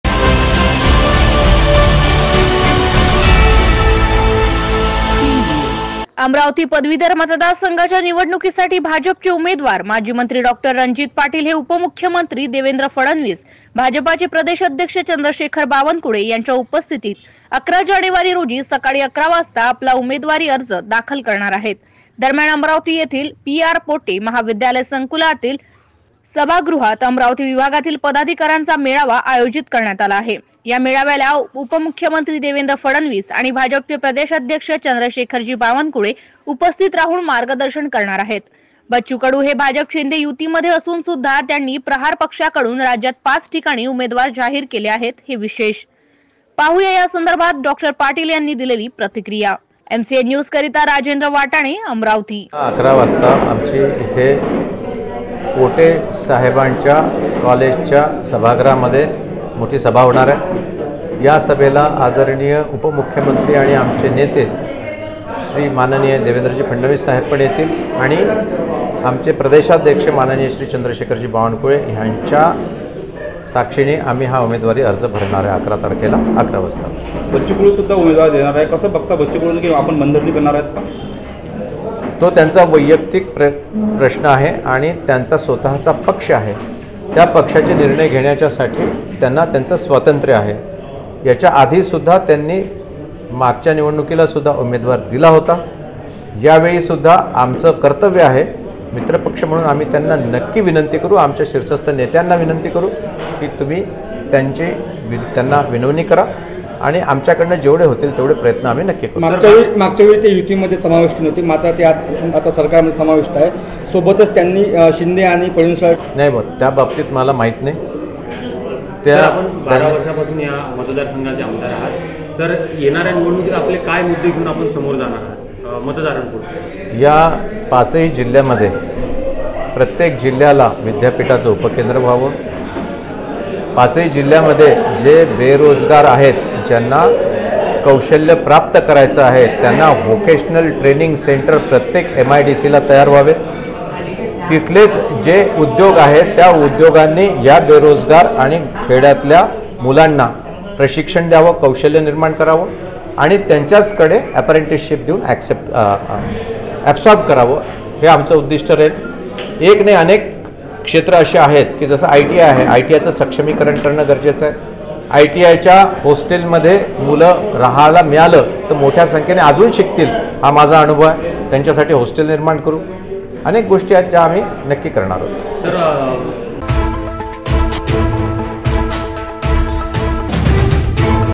भाजपाचे उमेदवार डॉ रणजित पाटील 11 जानेवारीला उमेदवारी अर्ज दाखल करणार..
MCNNEWSJALGANJAMOD. अमरावती पदवीधर मतदार संघाच्या निवडणुकीसाठी भाजपचे उमेदवार माजी मंत्री डॉ. रणजीत पाटील हे उपमुख्यमंत्री देवेंद्र फडणवीस, भाजपाचे प्रदेश अध्यक्ष चंद्रशेखर बावनकुळे यांच्या उपस्थितीत 11 जानेवारी रोजी सकाळी 11 वाजता आपला उमेदवारी अर्ज दाखल करणार आहेत. दरम्यान अमरावती येथील पी. आर.पोटे महाविद्यालय संकुलातील सभागृहात अमरावती विभागातील पदवीधरांचा मेळावा आयोजित करण्यात आला आहे.
या मेळाव्याला  उपमुख्यमंत्री देवेंद्र फडणवीस आणि भाजपाचे  प्रदेश अध्यक्ष चंद्रशेखरजी बावनकुळे  उपस्थित राहून मार्गदर्शन करणार आहेत..
बच्चू कडू हे भाजप शिंदे युतीमध्ये असून सुद्धा  त्यांनी प्रहार पक्षाकडून राज्यात पाच ठिकाणी उमेदवार जाहीर केले आहे हे विशेष.

पाहूया या संदर्भात डॉ पाटील
यांनी दिलेली प्रतिक्रिया..

एम सी एन न्युज करीता
राजेंद्र वाटाणे